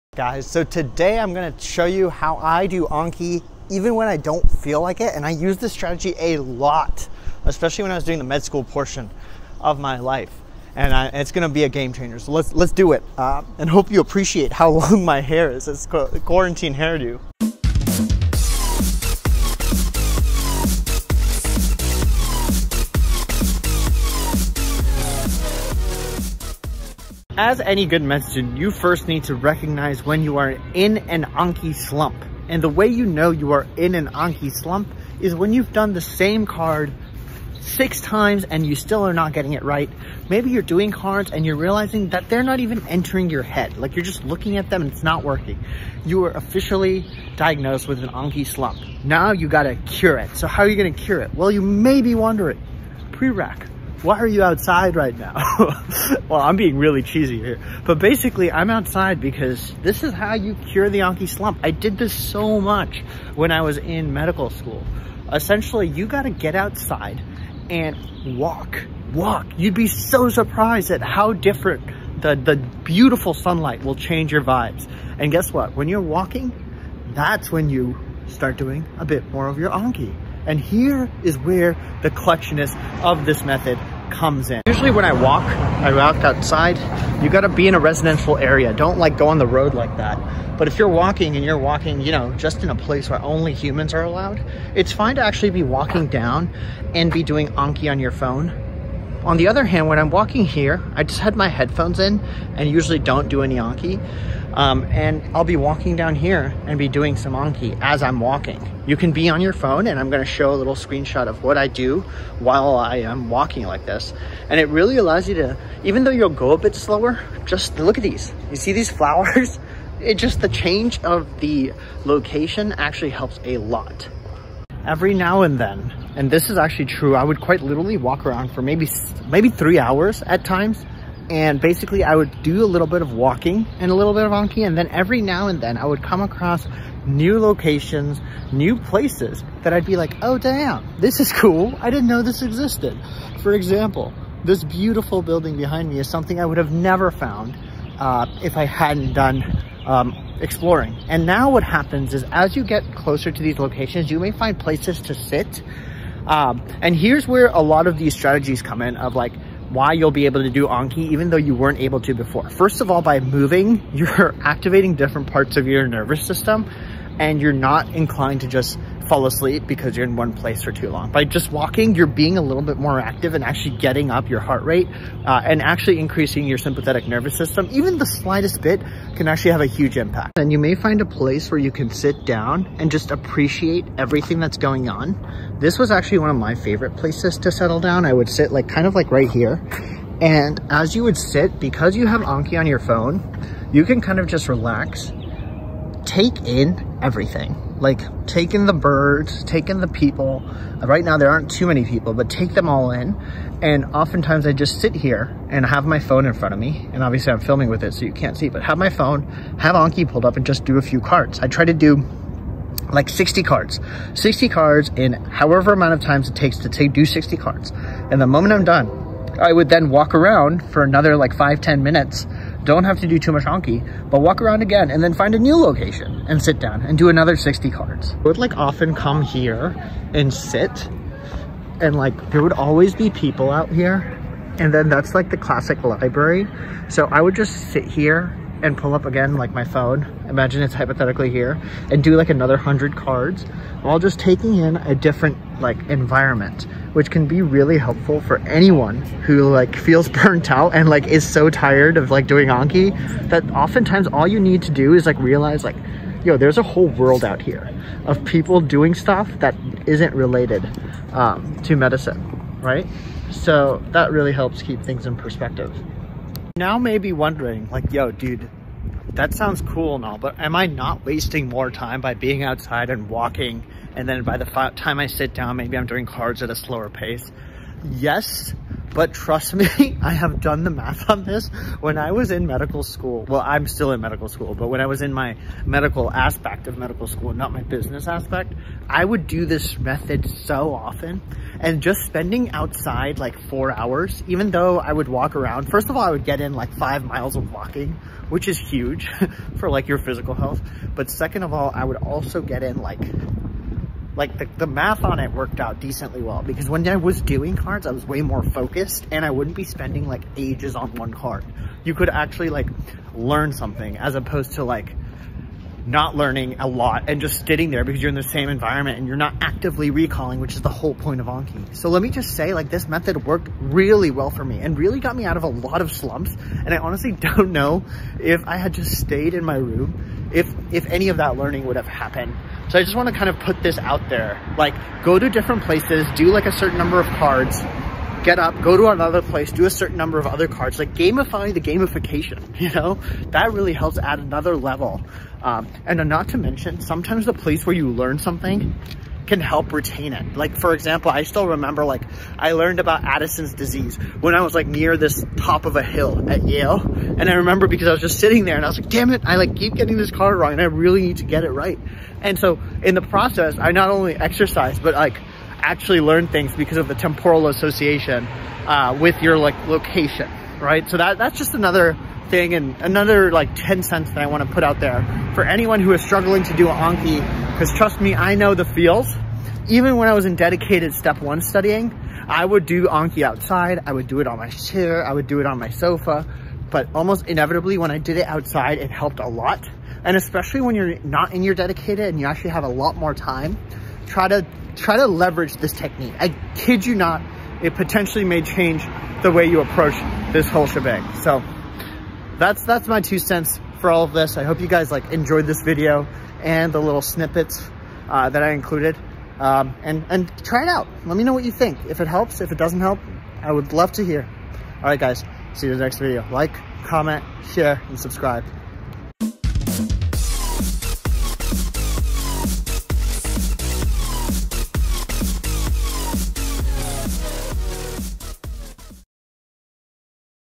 How to Battle ANKI-Fatigue
This personal strategy is one of the best ways to battle Anki fatigue.

00:00-00:18 - What the hell is my hair?

00:28-01:28 - What is an Anki-Slump?

01:28-07:11 - How to Overcome Anki Fatigue?